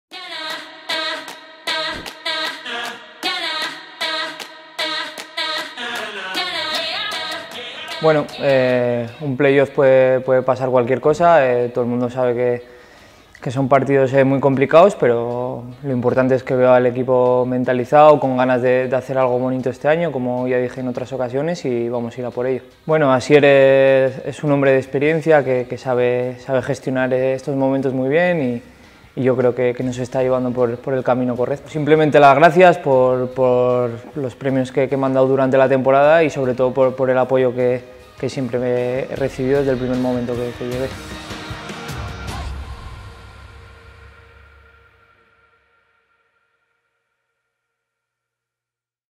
🏆 RUBÉN PARDO , elegido por la afición MEJOR JUGADOR de la TEMPORADA
Rubén Pardo, jugador del C.D. Leganés fue elegido por la afición pepinera como el mejor jugador de la temporada 2020/21.